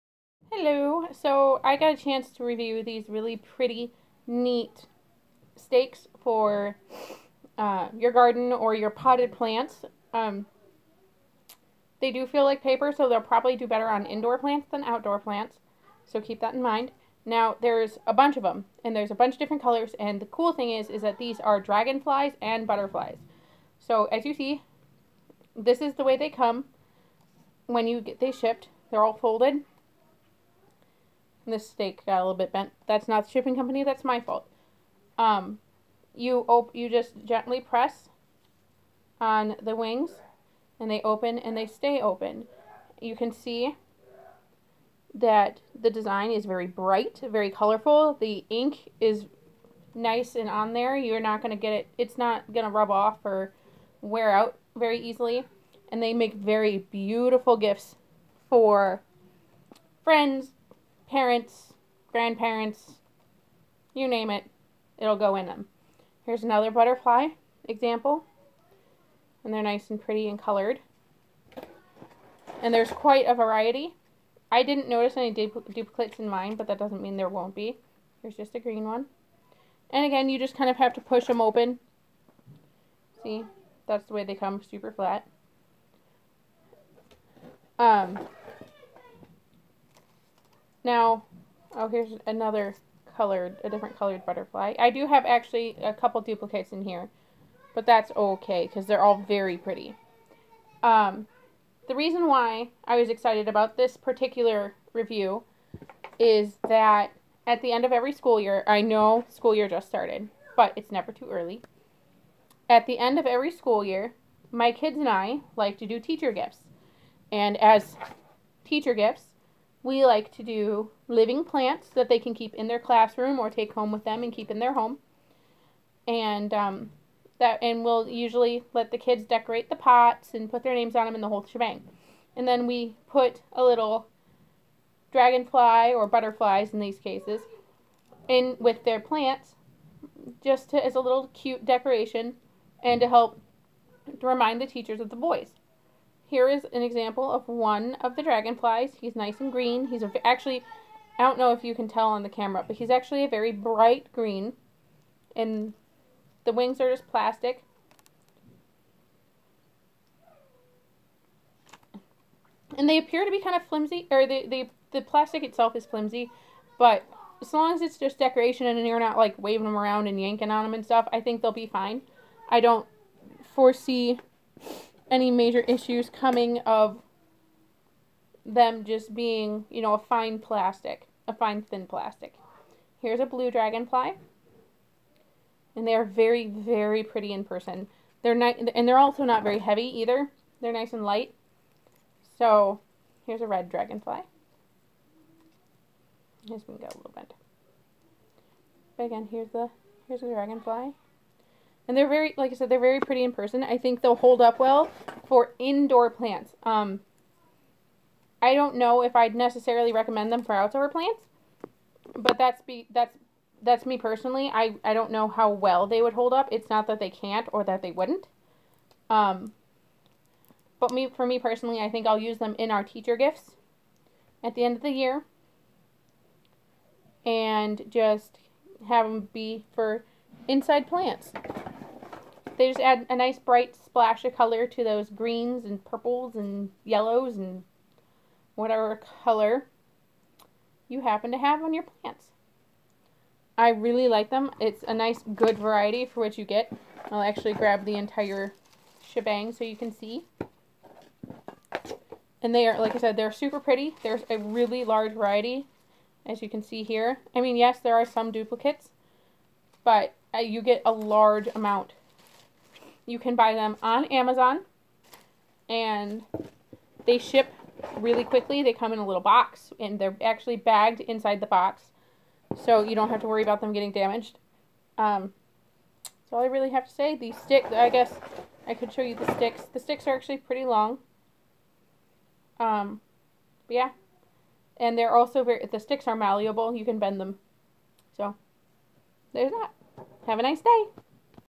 Dragonfly and butterfly garden decorations
Lots of variety and color perfect for indoor plants